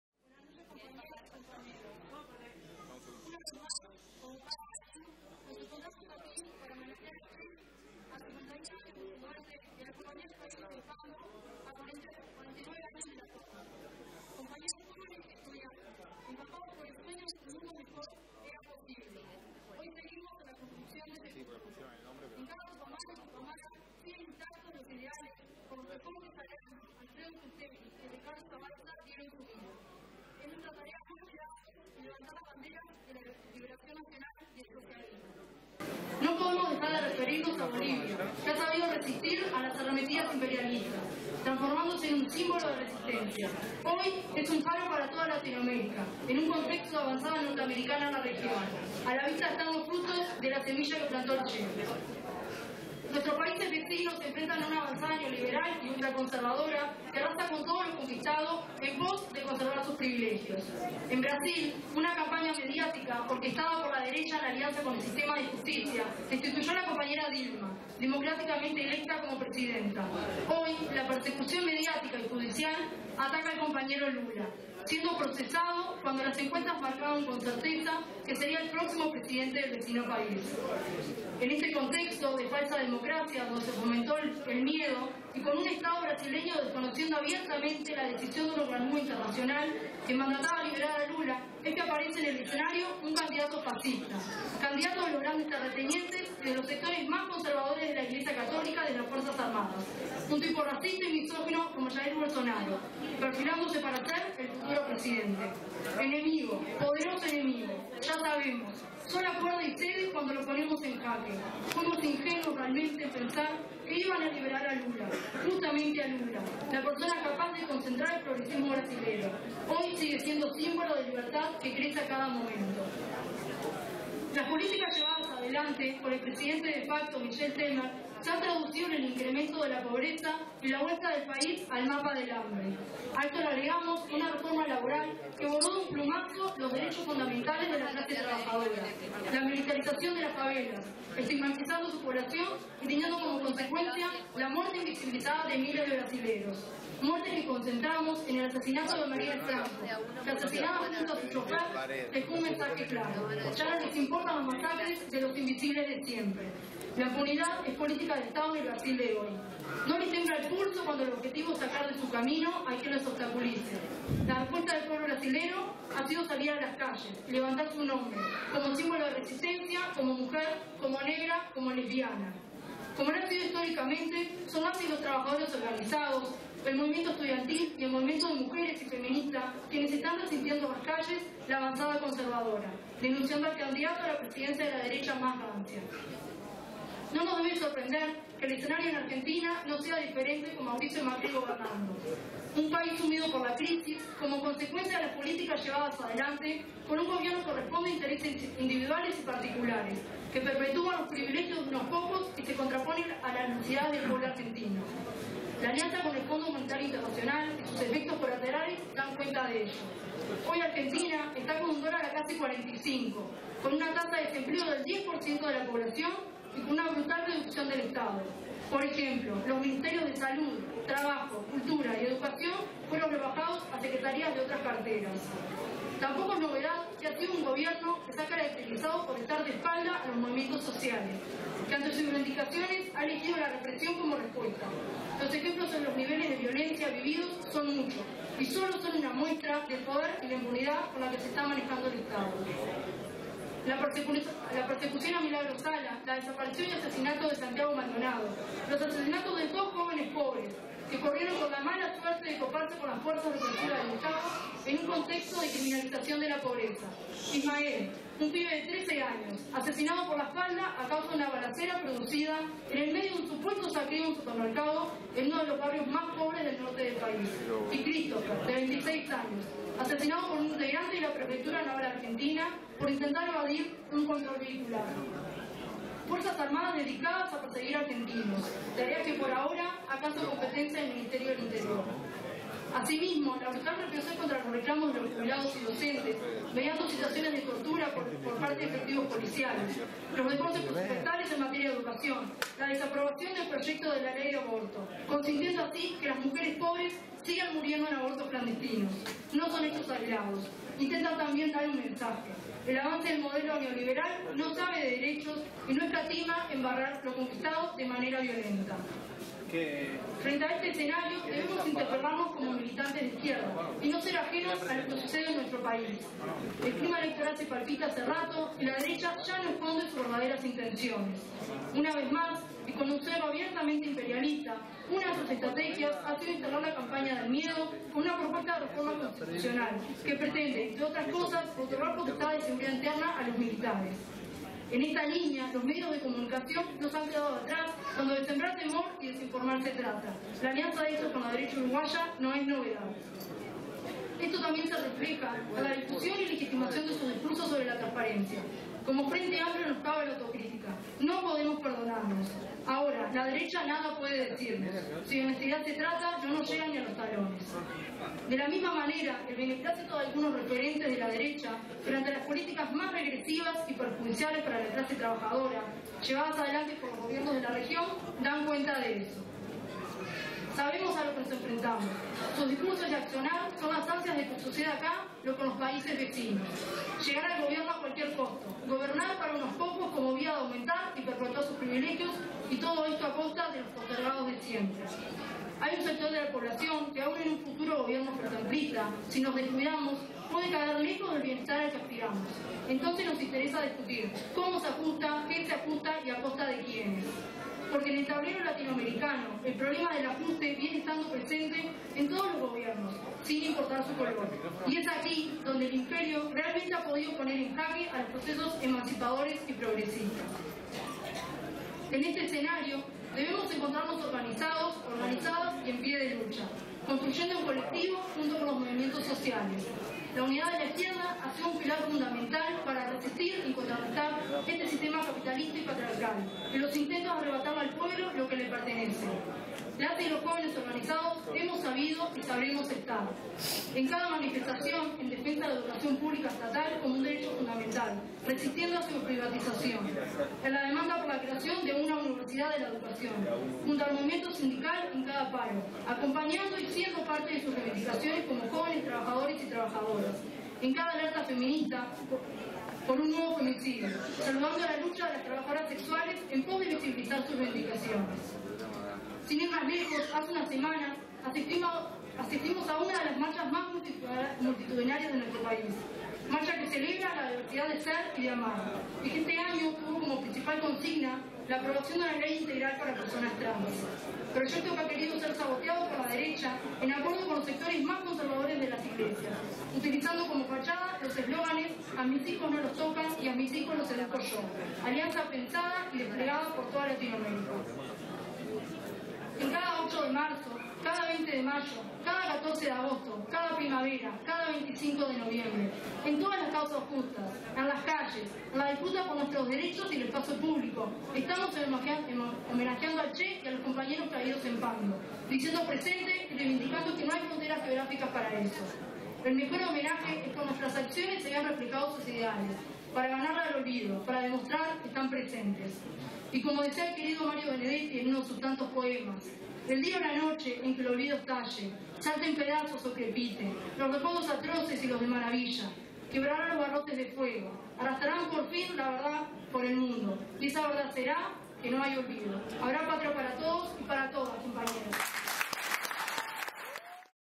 Fiorella Melo - Acto Tupamaro 2018
Discurso de Fiorella Melo del frente juvenil en el Acto Tupamaro "Por el Che y los caídos en Pando"